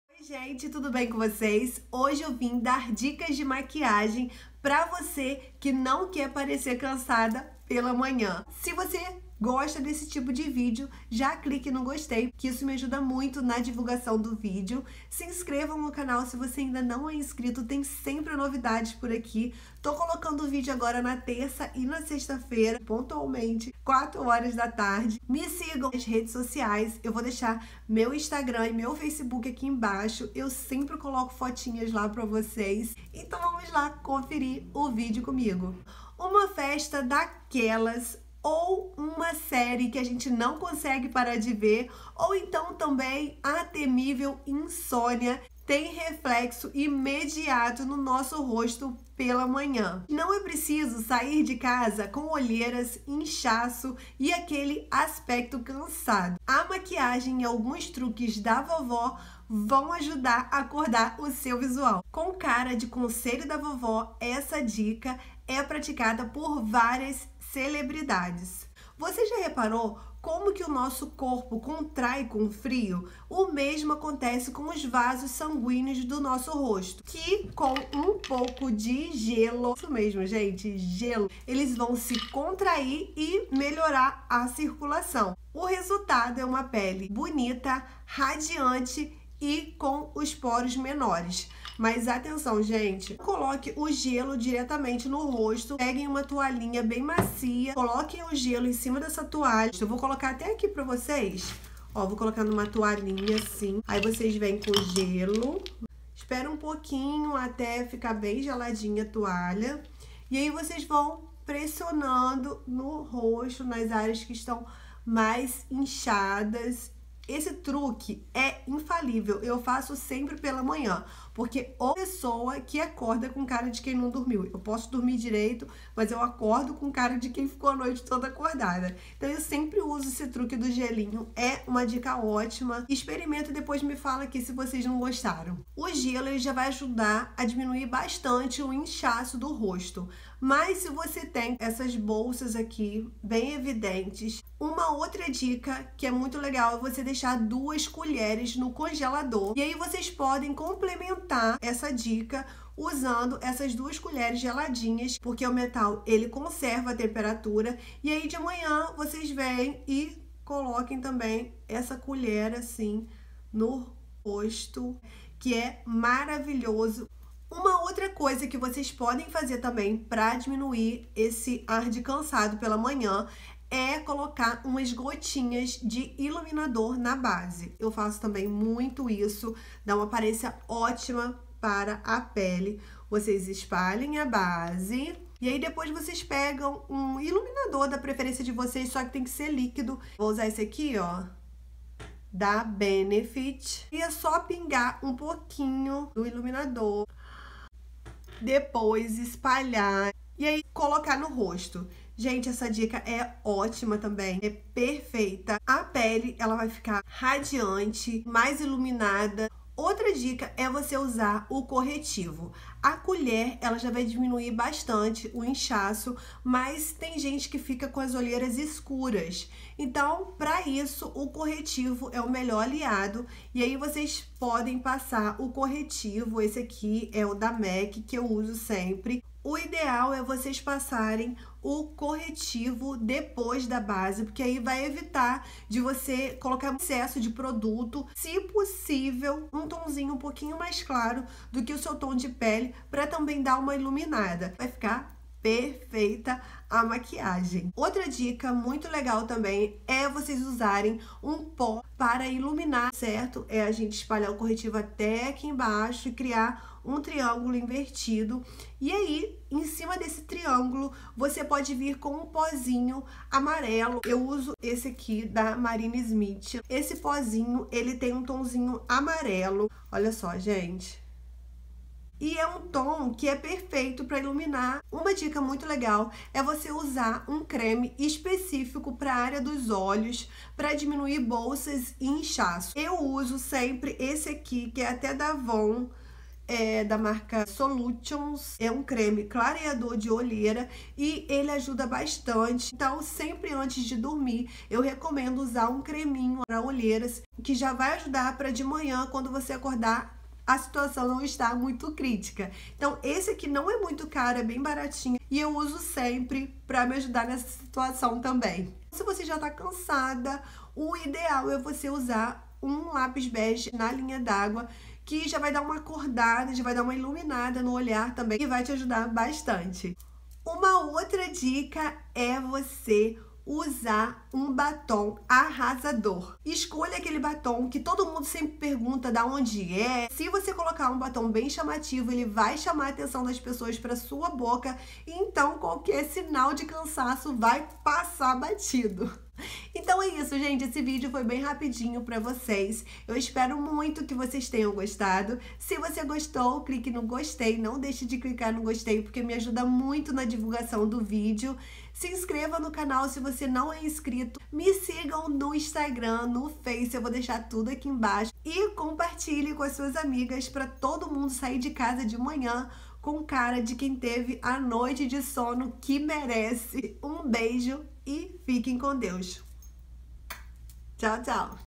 Dicas Incríveis Para Não Parecer Cansada de Manhã
O vídeo de hoje tem dicas de maquiagem e também da vovó para você não ficar com aparência de cansada pela manhã. Beijos ❤️

❤ Obrigada por me fazer sentir especial e amada. Você é especial para mim... Um beijo! 😘🙏🏻